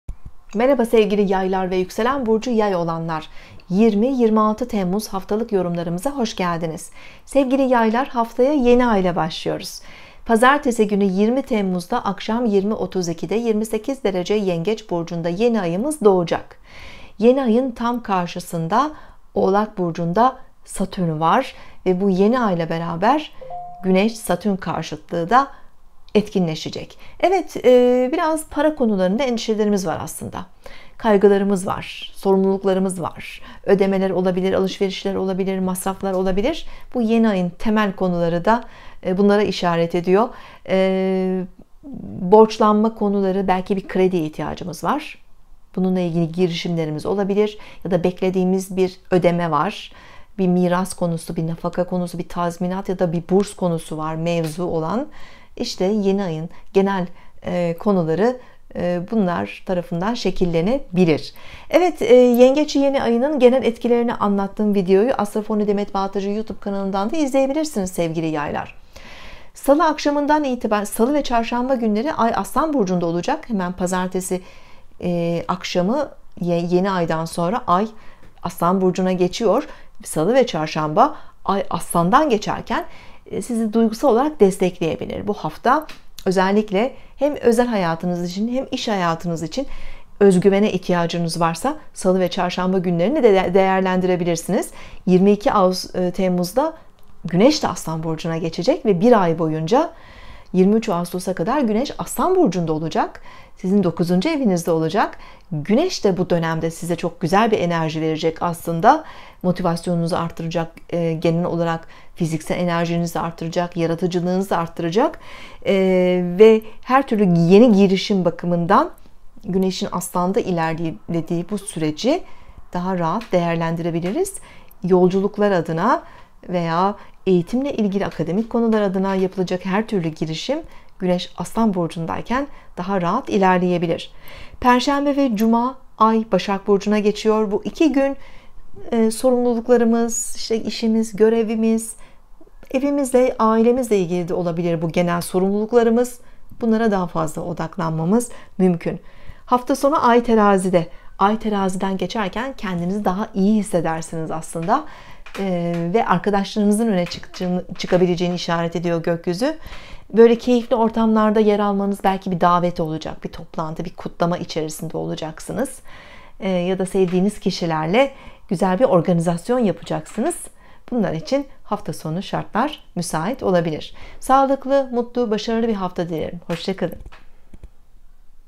Haftalık Burç Yorumları, Yay ve Yükselen Yay Burcu 20-26 Temmuz 2020 (Astroloji)
20-26 Temmuz 2020 Haftalık Burç Yorumları (Yay ve Yükselen Yay Burcu)
Yay ve Yükselen Yay Burcu 20-26 Temmuz 2020 haftası sağlık, iş, kariyer ve genel  haftalık burç yorumları (Astroloji)